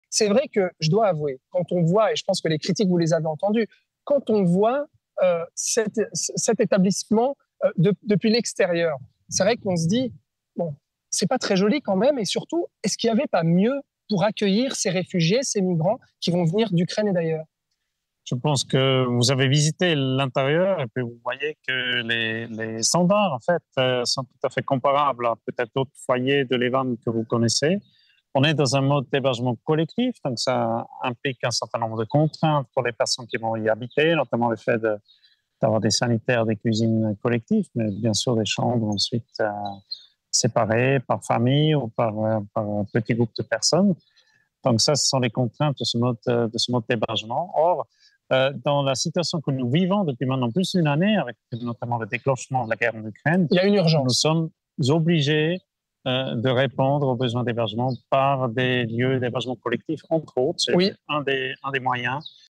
" Les standards sont comparables à d'autres foyers de l'EVAM "
Le foyer d’accueil temporaire de migrants s’apprête à ouvrir à Nyon, au quartier de La Levratte. Visite des lieux en compagnie des représentants de l’EVAM.